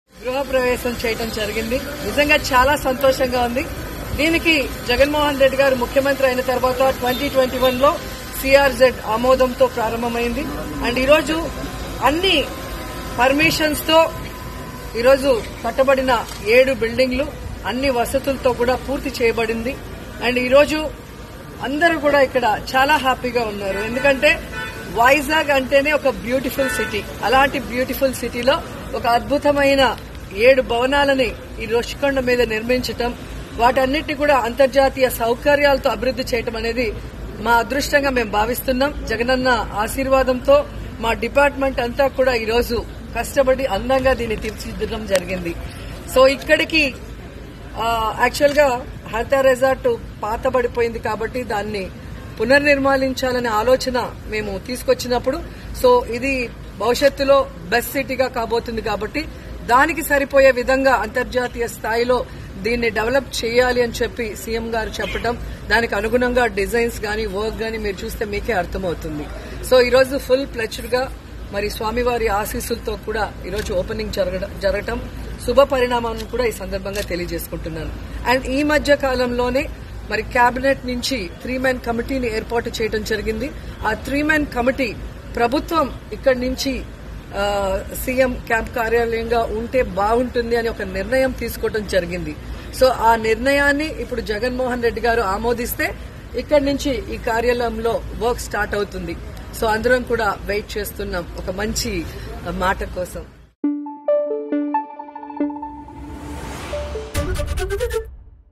Minister RK Roja Rushikonda Building | రుషికొండలో వివాదాస్పద భవనాలను ప్రారంభించిన మంత్రి రోజా | ABP
విశాఖ నగరంలోని రుషికొండ పై వివాదానికి కారాణమైన భవంతులను మంత్రులు ఆర్కే రోజా, గుడివాడ అమర్ నాథ్ ప్రారంభించారు.